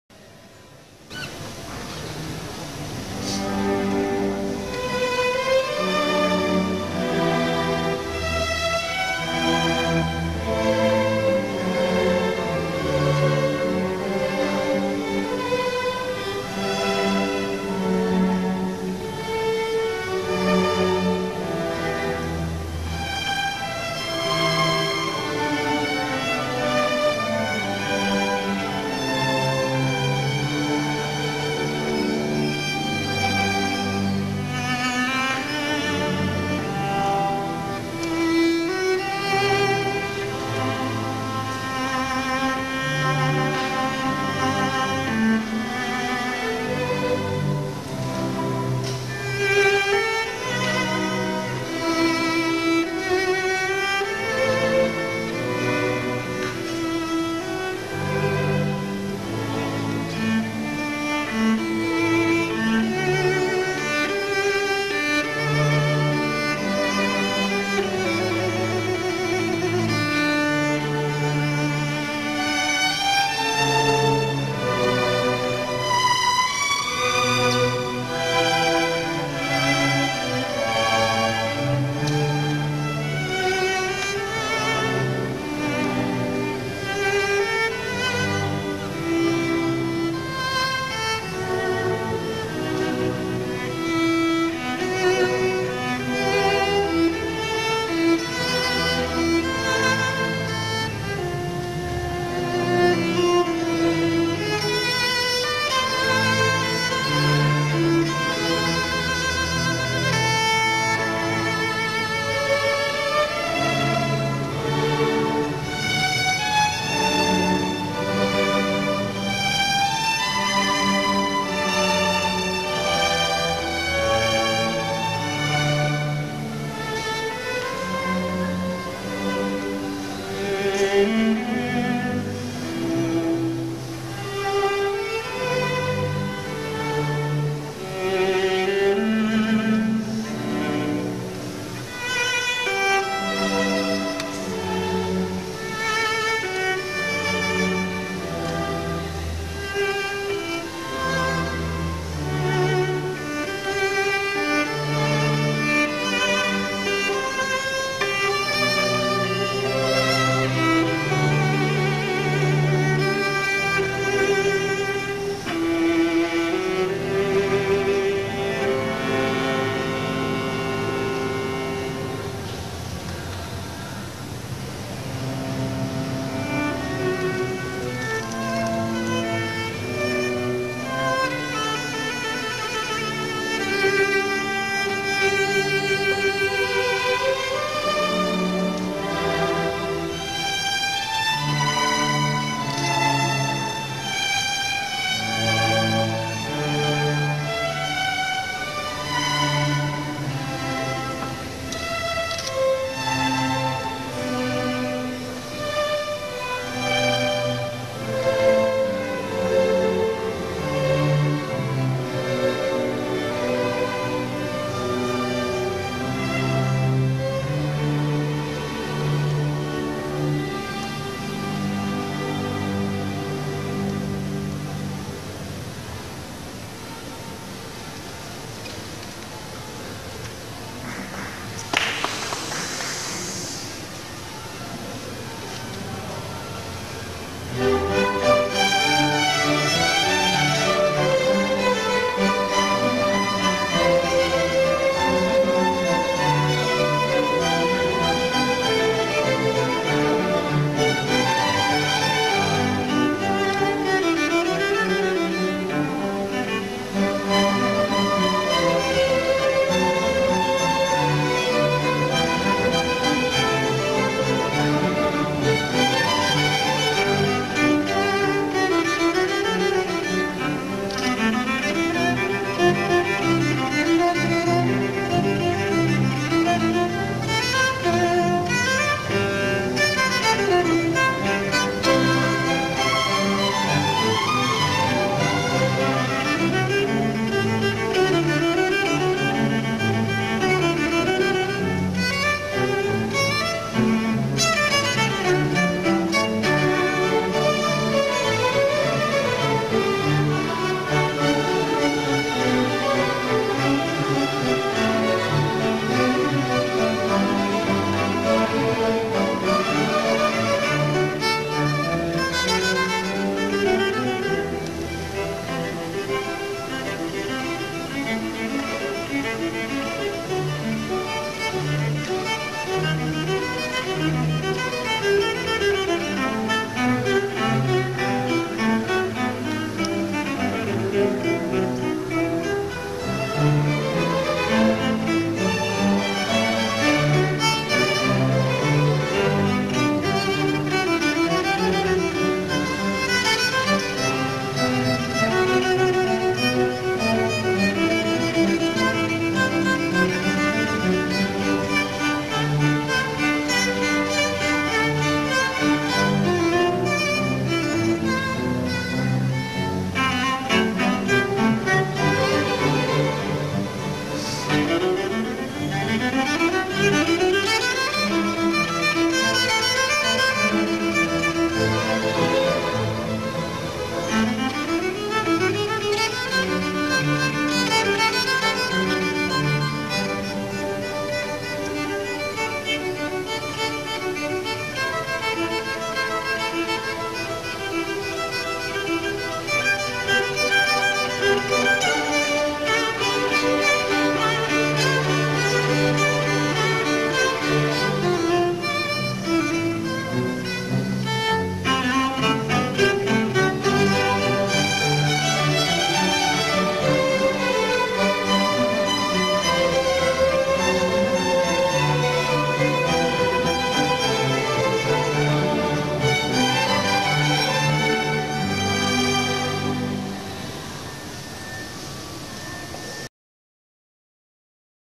TELEMANN 1er y 2do.mp4
el maestro joen vasquez y la juvenil de barquisimeto